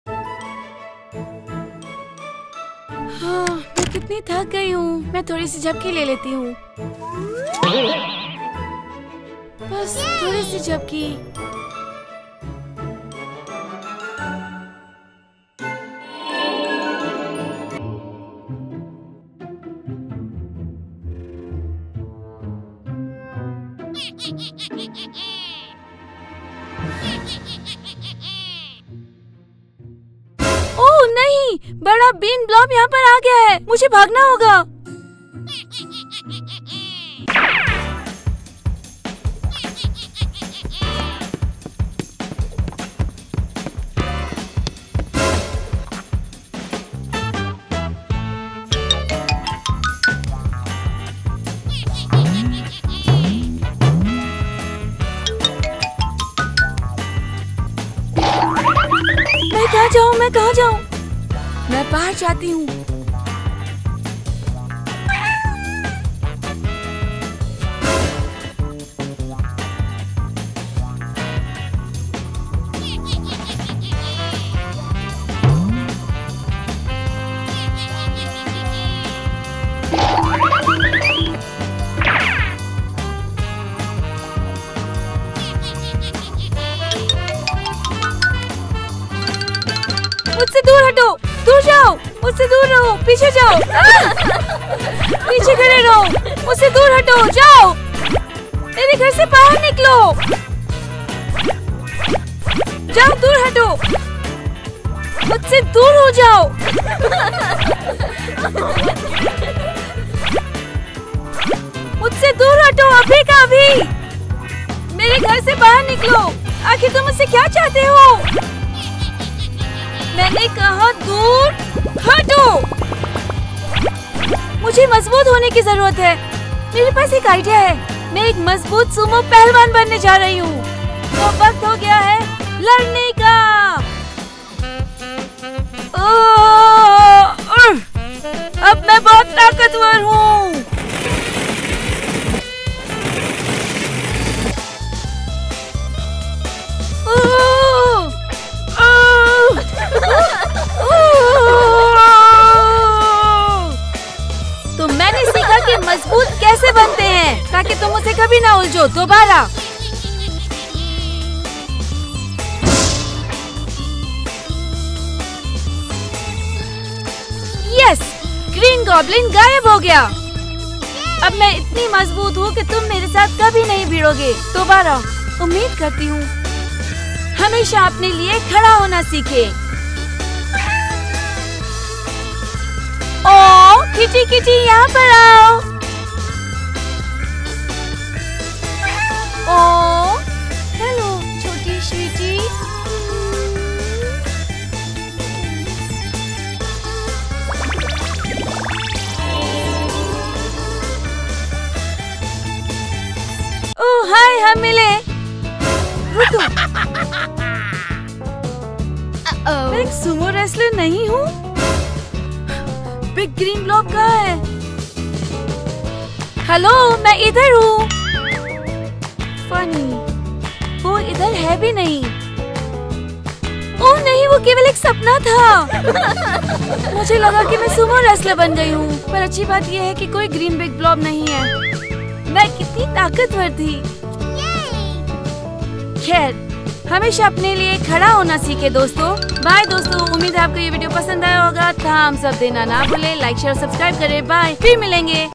हेदी हरे मुखौटे के साथ लुका-छिपी खेल रही है! Hide and Seek | Hindi kahaniya
ग्रीन मास्क वापस आता है और हेदी के साथ फिर से लुका-छिपी खेलता है। हेदी भी सूमो सूट में बदल जाती है और हरे मुखौटे को हरा देती है। क्या यह महज़ एक सपना था? Hide and Seek | Hindi kahaniya

Join the AIR Creators Ecosystem with 3000+ channels and monetize your channel better!